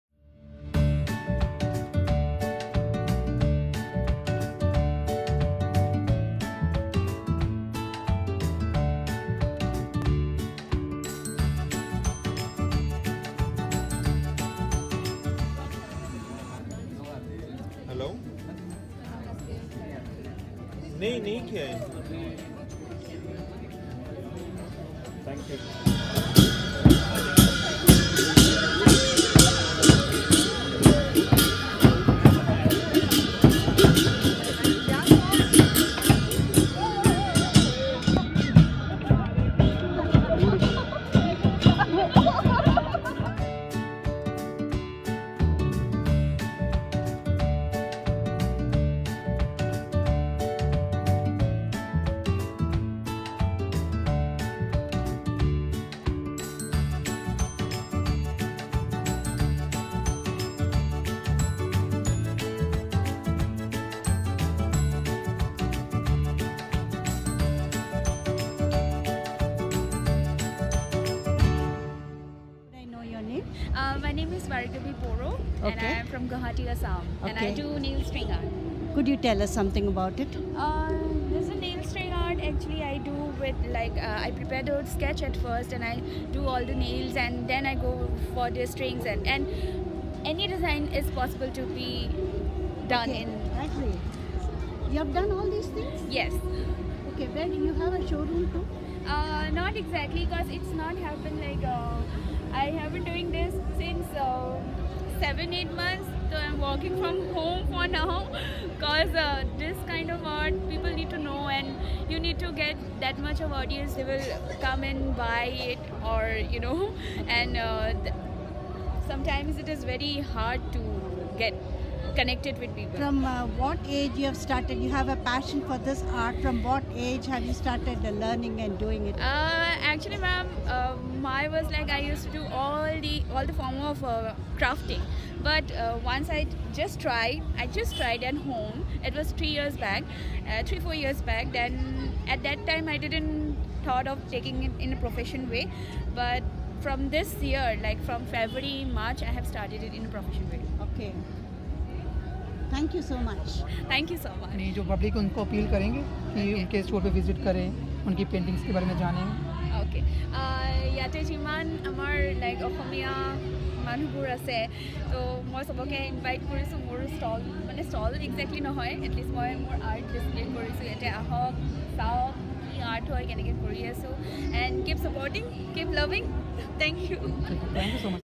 Northeast Festival Delhi 2019 -Nail String Art
This artists work was very much appreciated by the visitors. The artist is gifted,very creative, her themes are also interesting.